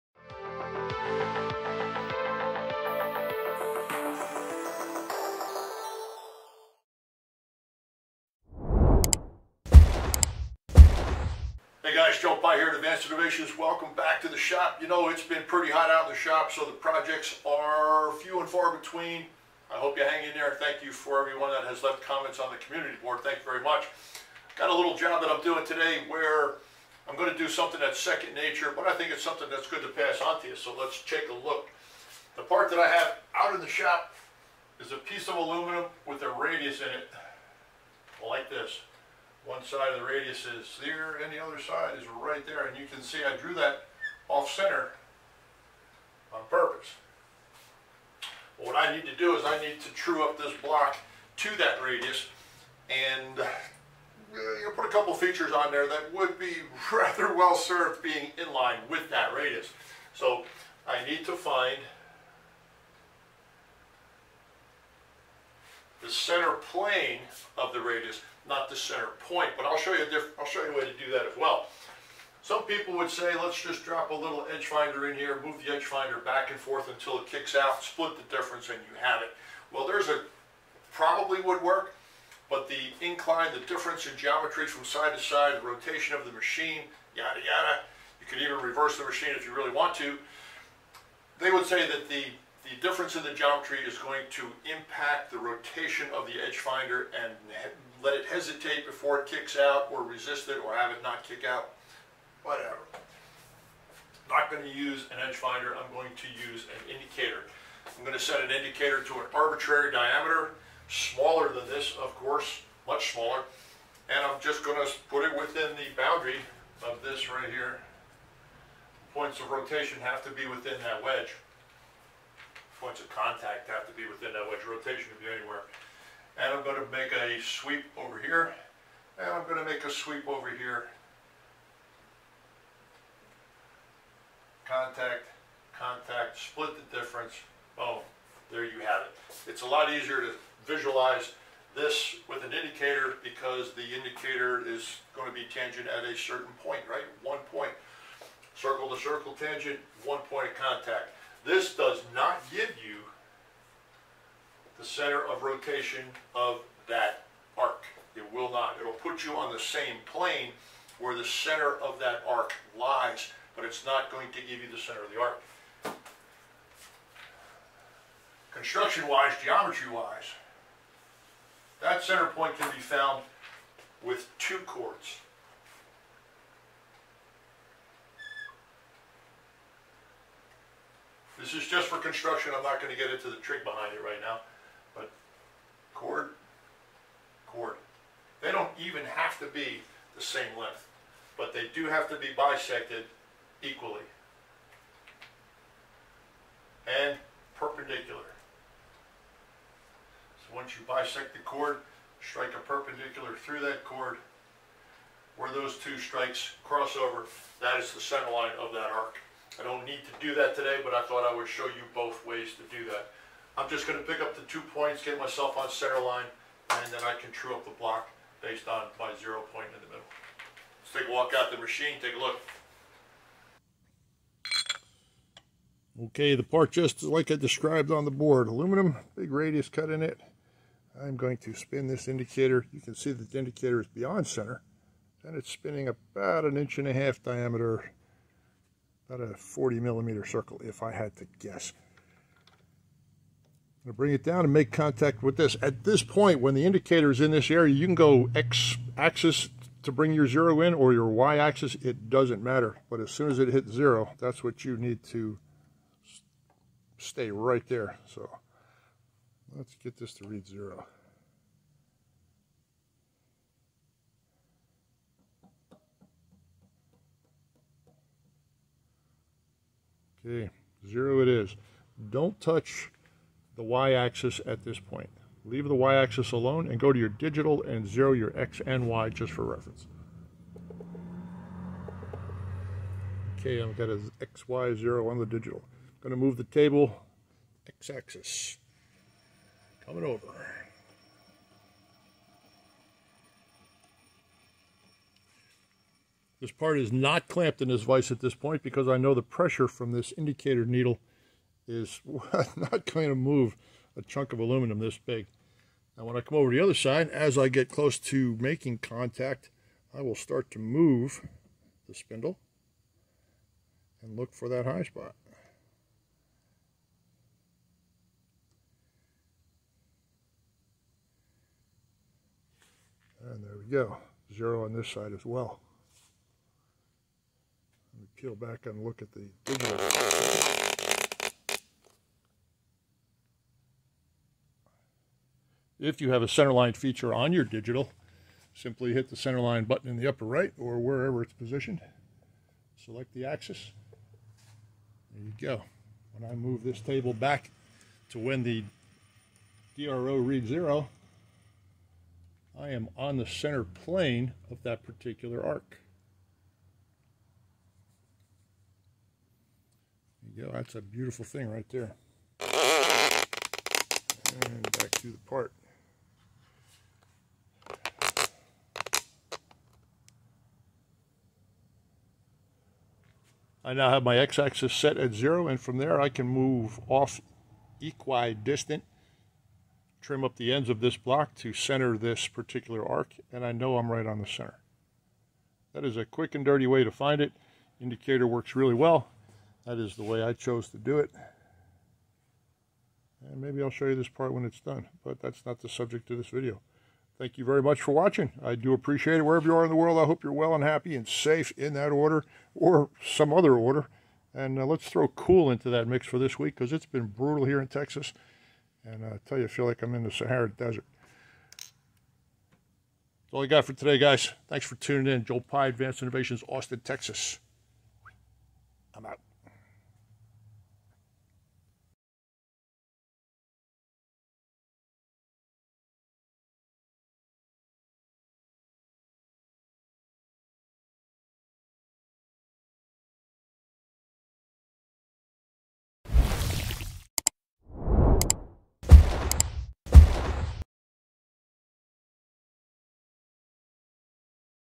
How To Quickly Find the Center Plane of an Arc......
This video demonstrates an easy way to find the center plane of an arc or curved surface. From this position, finding the center point is much easier. Its quick but valuable. Take a look !
Blue Magic metal polish
B&L Eye Loupe
Indicol 178
Postage Scale